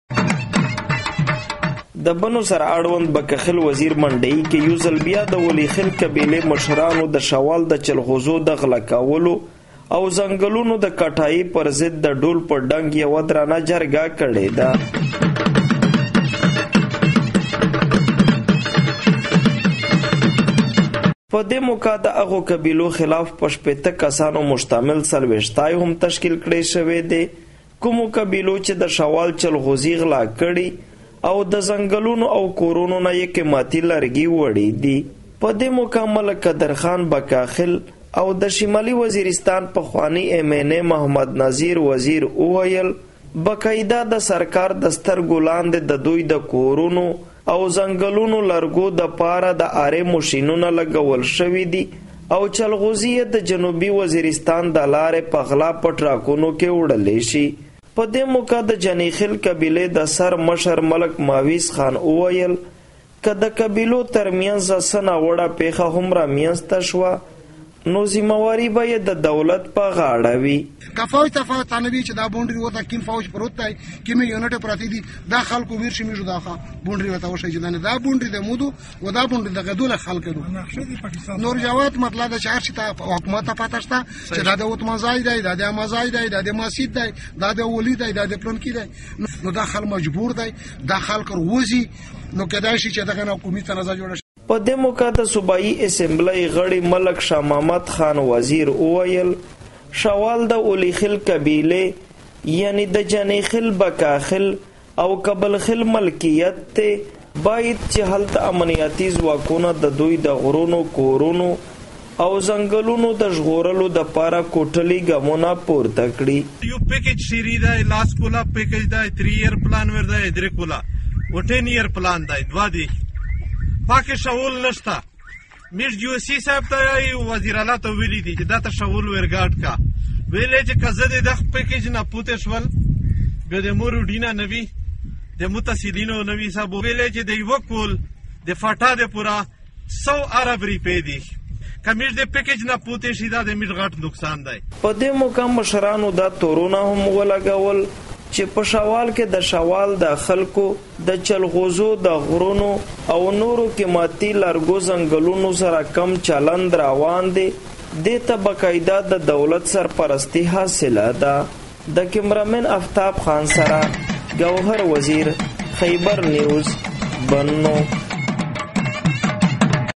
د شمالی وزیرستان شوال حفاظت دپارہ ولی خیل وزیر قبیلی څلويشتے تشکيل کړو
بنو بکاخیل پۀ مقام د ولی خیل وزیر قبیلې مشرانو د شوال د چلغوزو ژغورلو او د ځنګلونو نه د قیمتی لرګو د غله کولو پرضد قامی څلویشتے جوړ کړے دے
او تور یې لګولے دے چې چا هم د شوال نه د کروړونو روپو چلغوزی پۀ غله شوکولي او د ځنګلونو او کورونه یې قیمتی لرګی وړی دی.
پۀ دې کښې سرکاري خلق برابر ملوث دے.
نور تفصیل د ګوهر وزیر پۀ دې رپورټ کښې.

ټیکسټ

بنو کښې بکاخیل، جانی خیل او کابل خیل قبیلو د شوال د حفاظت د پاره پخپله څلویشتے جوړ کړے دے

جرګې تور لګولے چې د دوي د چلغوزو غرونه پۀ غله شوکول شوی او د ځنګلونو قیمتی لرګی یې د سرکاري خلقو پۀ ملاتړ غله کولې شي.

څلویشتی پرېکړه کړېده چې کومو قبیلو شوال ته توان وړ اړولے دے د هغې نه به بدل اخیستلے کېږي